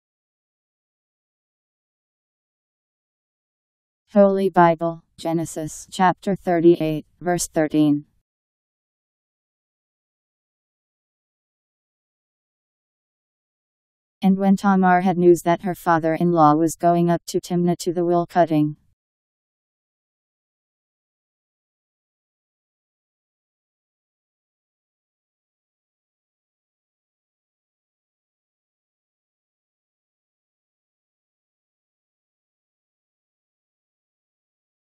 Holy Bible Genesis Chapter 38 Verse 13
Video shows Holy Bible Genesis Chapter 38 Verse 13: And when Tamar had news that her father-in-law was going up to Timnah to the wool-cutting, Powered by MaryTTS. Text taken from Bible in Basic Edition